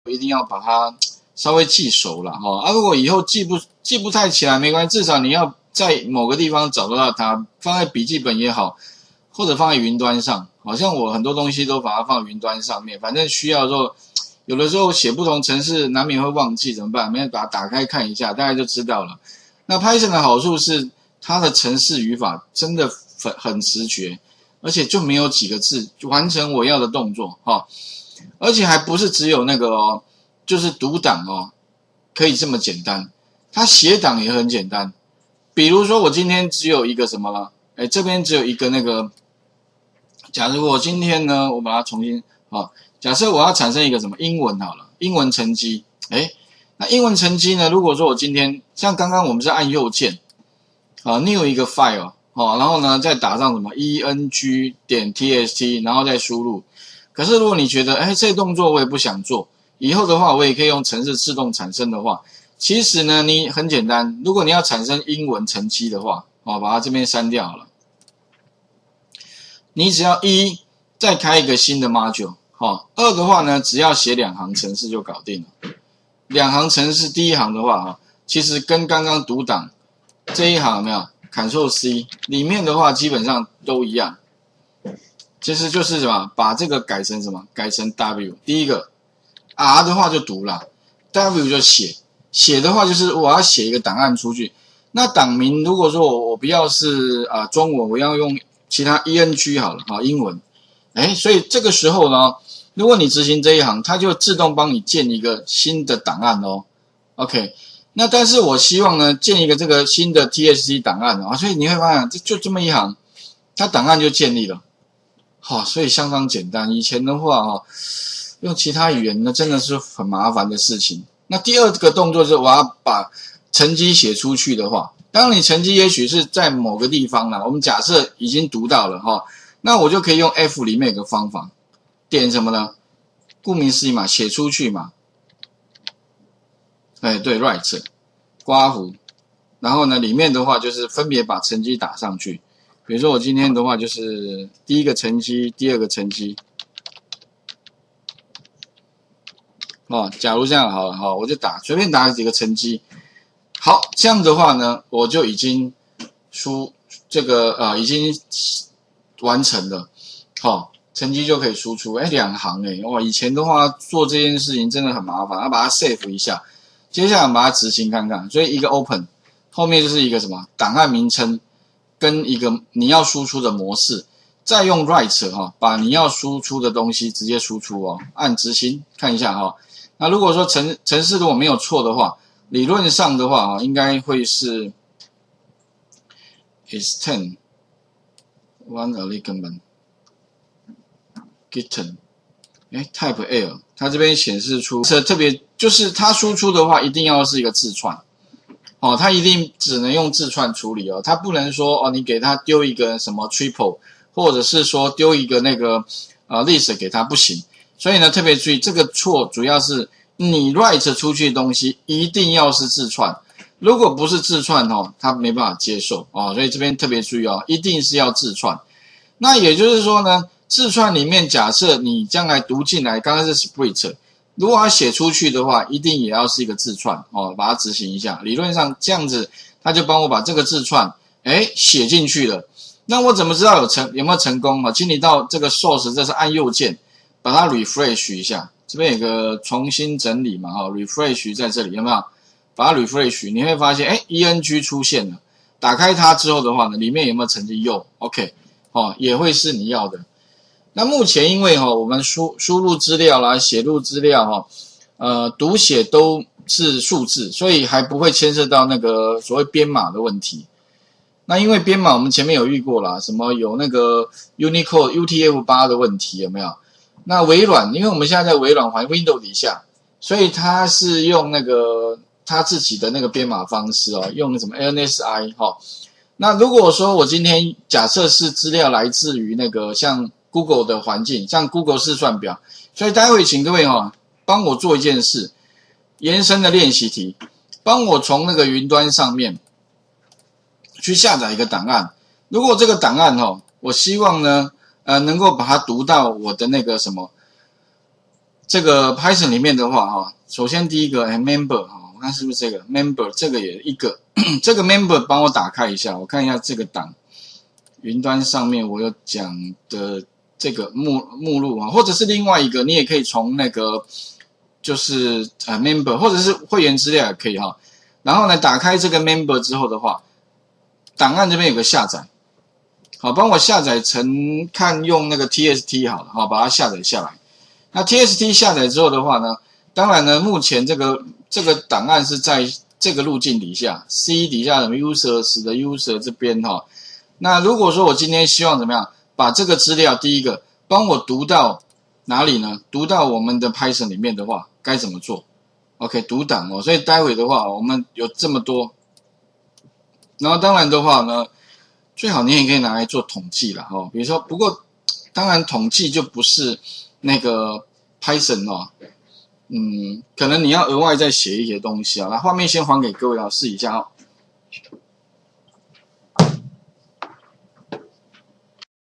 05 成績寫到文字檔中
從EXCEL VBA自動化到Python程式開發入門第8次練習題MonthCAI&範例猜數字1到20猜一個數字&如何將PYTHON轉換為VBA&VBA重要函數到Python與只能猜3次&範例猜拳小遊戲)

上課內容：
01_重點回顧與練習題MonthCAI
02_範例猜數字1到20猜一個數字
03_如何將PYTHON轉換為VBA說明
04_VBA重要函數到Python與只能猜3次
05_範例猜拳小遊戲

檔案處理
資料庫處理

上課用書：
參考書目

作者：葉難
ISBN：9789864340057
出版社：博碩文化
出版日期：2015/04/02

吳老師  107/4/26